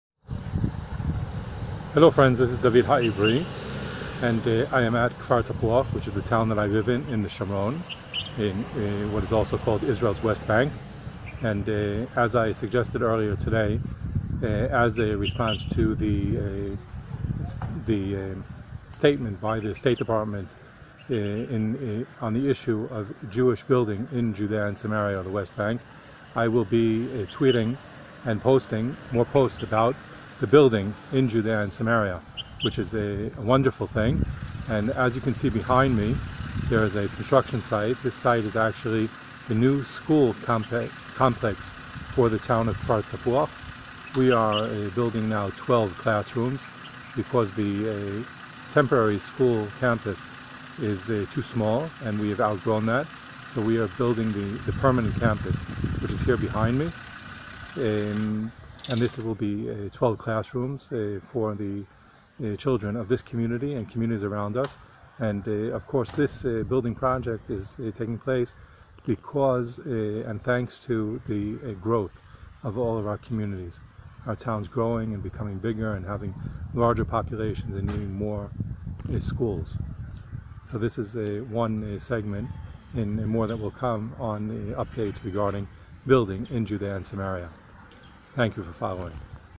Jewish Building Goes on in Israel's West Bank - a New School for Kfar Tapuach
The US State Department is not happy about Jewish people living in the Heartland of Israel. I think that every new home, school and playground is something to celebrate. Thank God that we are here in the ancestral homeland of the Jewish people. If the US State Department doesn't like it, they should look the other way.

Short video report from a Shomron Day Experience with visiting Rabbi Shmuley Botach and his family.

Our guests had a chance to visit factory in the Barkan Industrial Park that is being targeted by the BDS movement which campaigns around the world to boycott Israeli products manufactured in Judea and Samaria AKA "The West Bank".

They spoke with Palestinian workers and heard their opinions on the BDS movement that aims to keep them without work and means to provide for their families.

We had a great dairy lunch at Cafe' Tov - Good Coffee at Kfar Tapuach.

And enjoyed an amazing inspirational visit to Mitzpeh Yitzhar and it's Chabad Yeshiva with Rabbi Yitzik Sondroi and Israeli Media Personality Ezri Tubi.